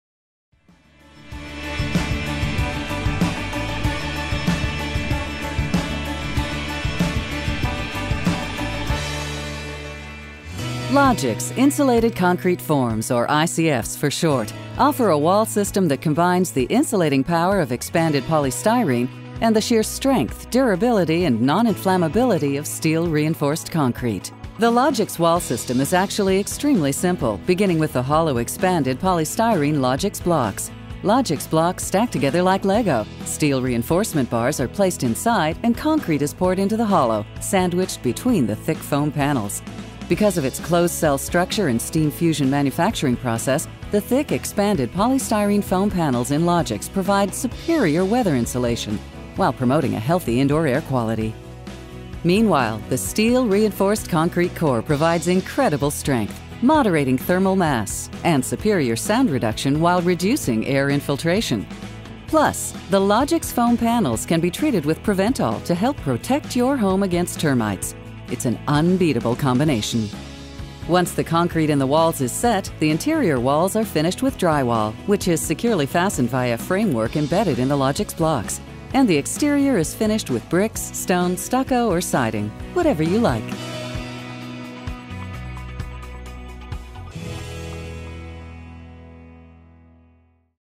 How Logix ICF Works
Logix Insulated Concrete Forms is the complete construction solution for homes and buildings. Turning simplicity into a science, the straightforward assembly of Logix ICF is easy to learn — and even easier to put into practice — and its design flexibility is an architect's dream. See for yourself!